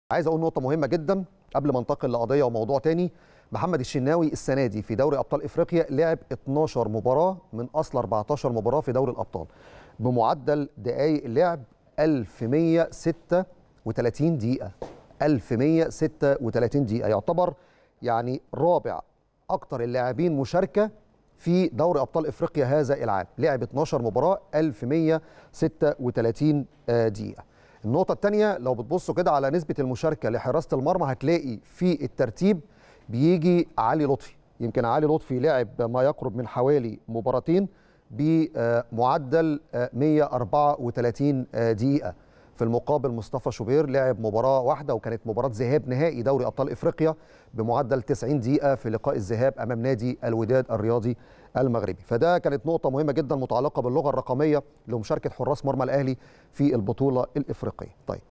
سعيد:الشناوي خاض 12 مباراة في دوري الأبطال بمعدل 1136 دقيقة ويعد رابع أكثر اللاعبين مشاركة هذا العام
محمد سعيد: محمد الشناوي خاض 12 مباراة من أصل 14 في دوري الأبطال بمعدل 1136 دقيقة.. ويعد رابع أكثر اللاعبين مشاركة هذا العام